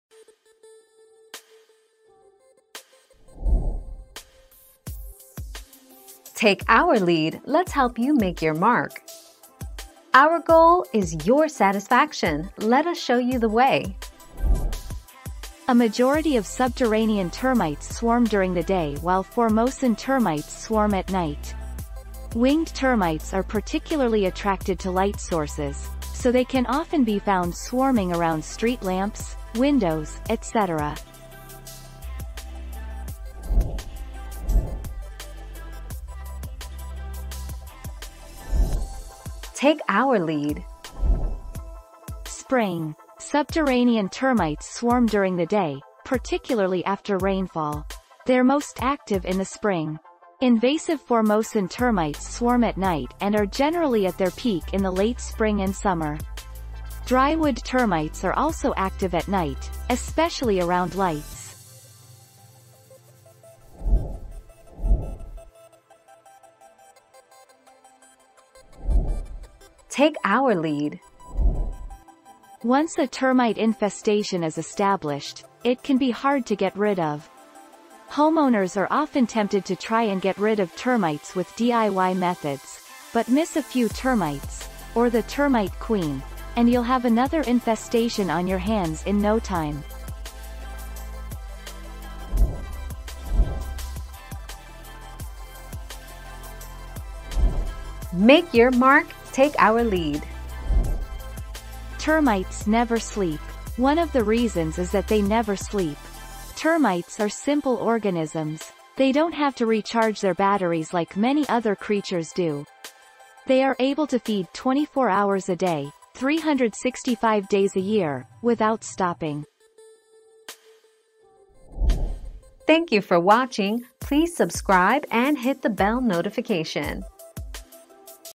Are termites attracted to light at night?
00:00 - Are termites attracted to light at night?
00:35 - What time of day are termites most active?
01:05 - Are termites hard to get rid of?
01:33 - Can you sleep in a room with termites?

Laura S. Harris (2021, June 3.) Are termites attracted to light at night?
    AskAbout.video/articles/Are-termites-attracted-to-light-at-night-250719